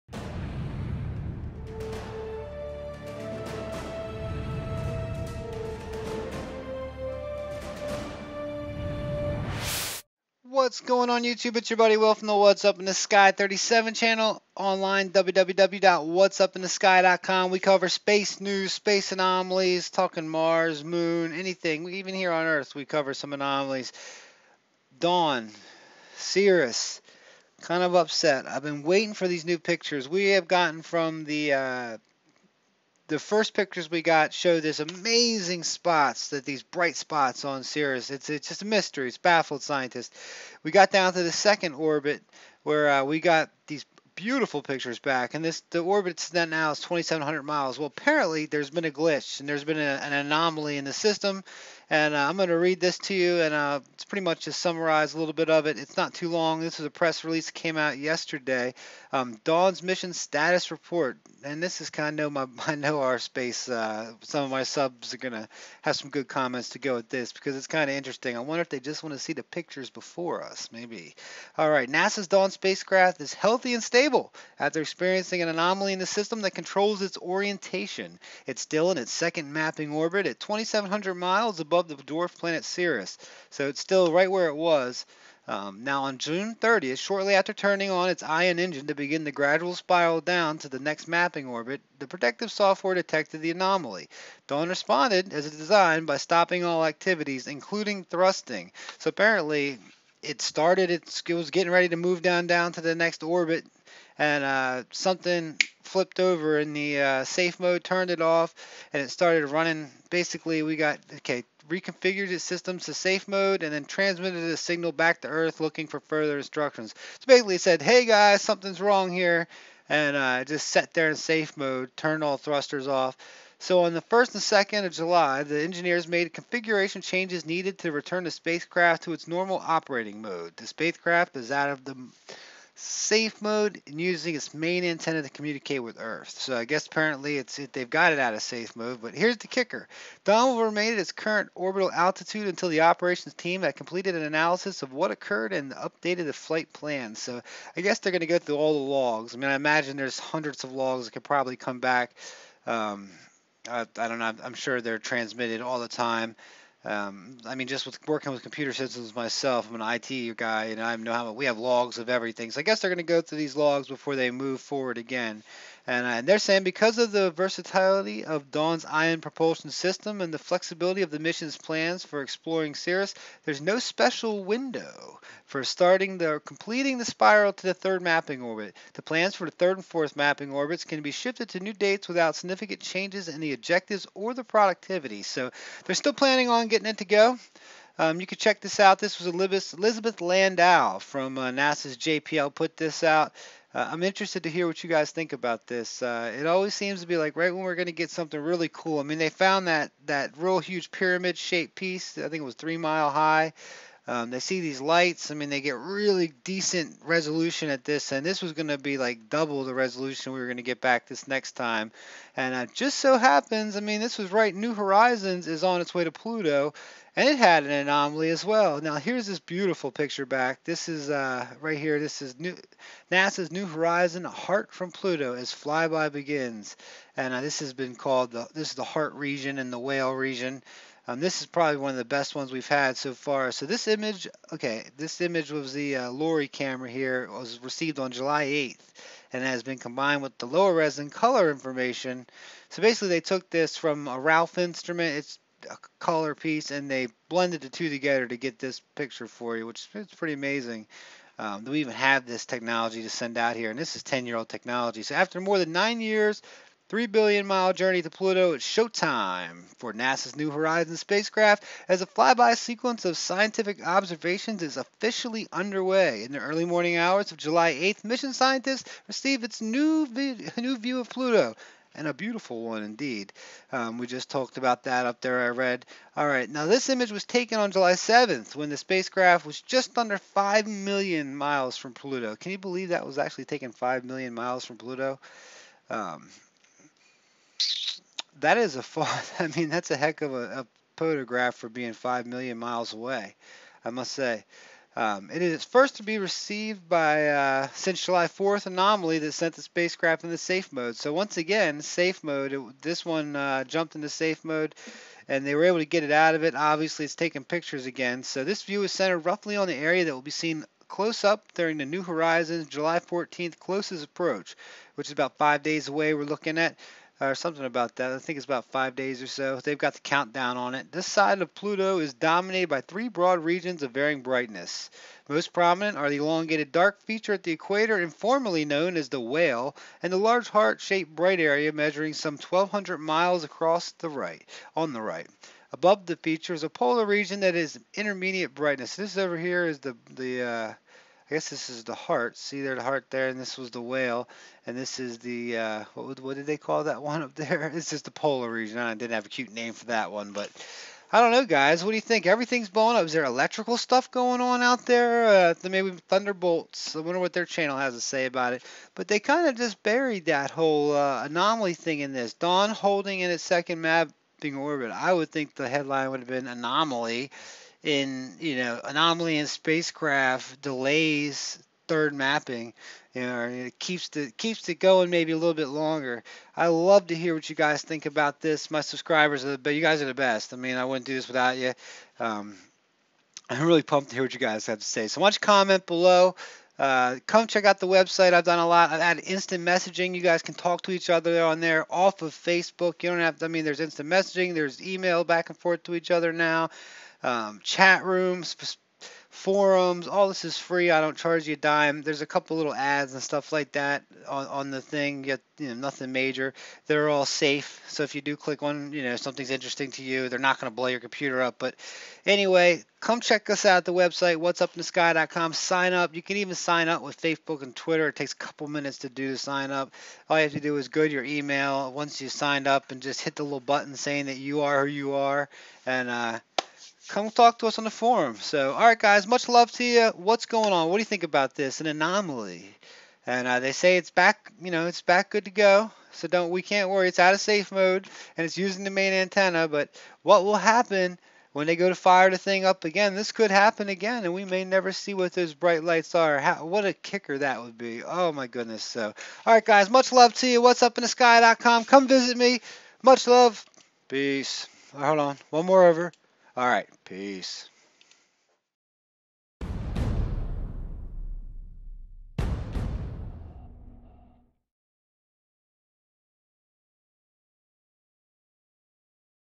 Dawn Spacecraft Suffers "Glitch" Moving Orbits Over Ceres, New Pluto Image! WUITS Space News
I was eager today to see if any new images have returned to Earth from NASA's Dawn spacecraft and to my surprise I saw this press release entitled "Dawn Holding Second Mapping Orbit" (linked below) in which they totally buried the true headline.  Dawn suffered an Anomaly when it went to move from its current orbit around Ceres to its third mapping orbit.  This anomaly even shut down the thrusters according to the press release.  This came on the heals of the New Horizons glitch on July 4th which sent it into safe mode.  So many anomalies later I am surprised the headline did not point out the anomaly.  Dawn should have reached the Third Mapping Orbit in August so we will see how far off we get there from now. I really want to hear what you subscribers think about this one!